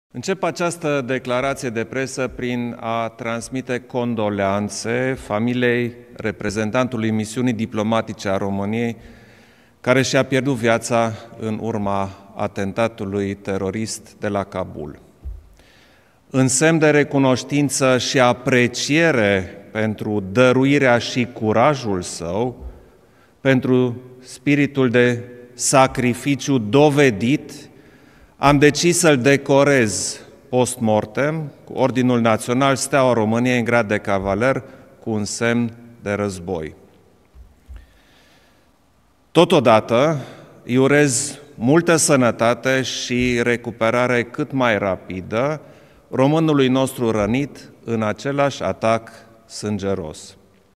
Mediafax 04 Septembrie- Iohannis:Am decis să-l decorez post-mortem pe diplomatul român ucis în Kabul
Mediafax 04 Septembrie:  Președintele Klaus Iohannis a decis să-l decoreze post-mortem pe diplomatul românul ucis la ambasada României de la Kabul cu ordinul național Steaua României în grad de cavaler cu însemn de război.